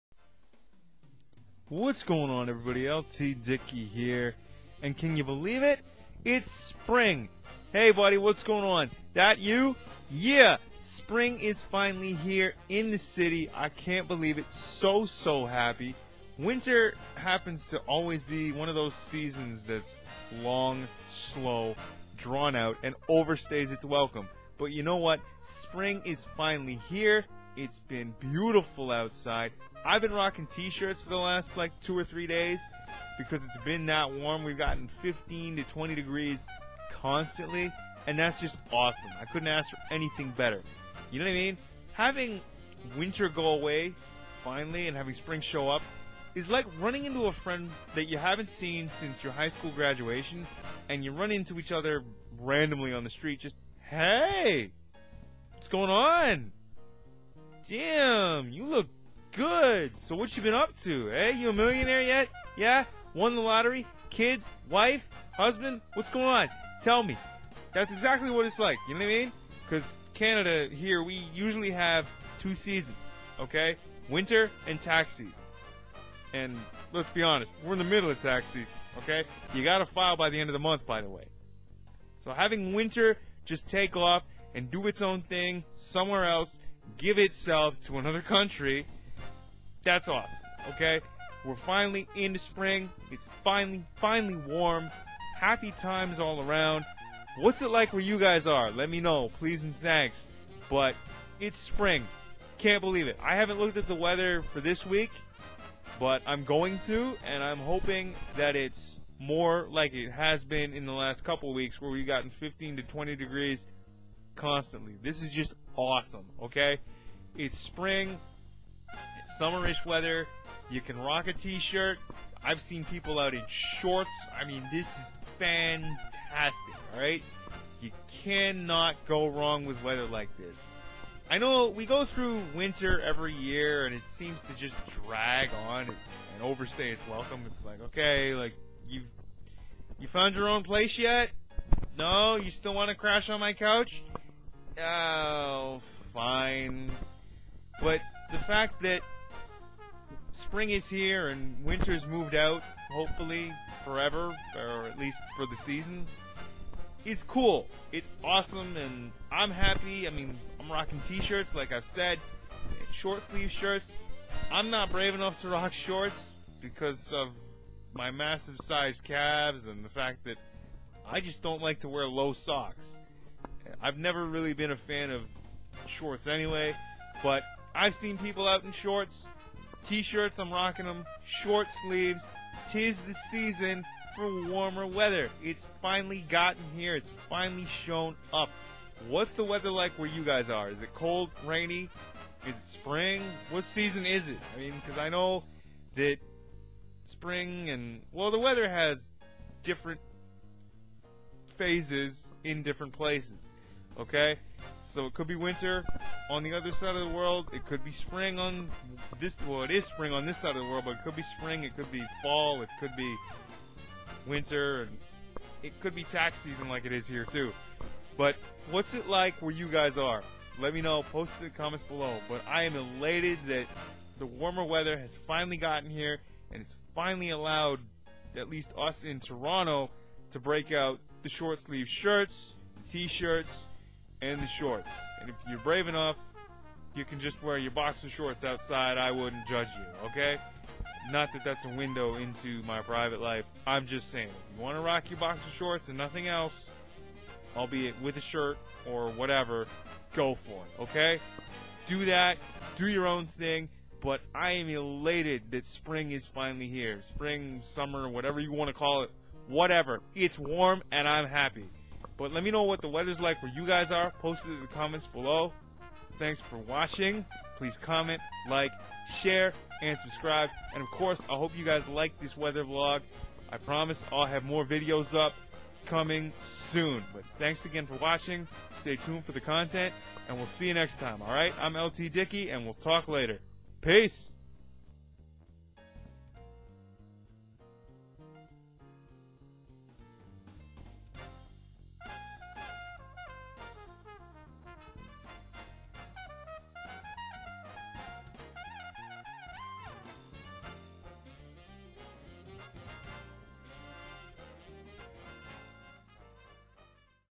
Hey Spring, Is That You?! - Weather Vlog #32
What's up everybody? We're well into April. Winter has been hanging on for as long as it can, but I think it has finally moved on! Oh, happy day! I've seen some awesome weather over the past couple weeks, 15 to 20 degrees everyday easily! It's fantastic! T-Shirt and shorts! Can't ask for anything better! What is the weather like where you are? Let me know!